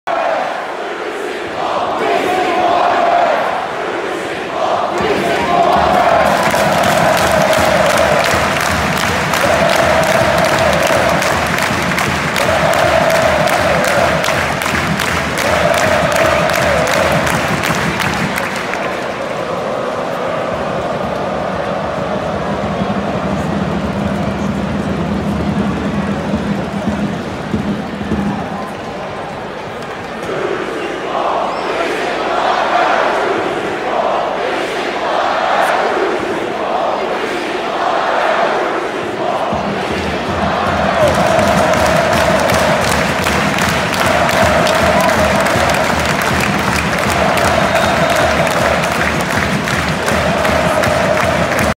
Western Sydney Wanderers VS Sydney FC 2021 3-2
Red and black Bloc celebration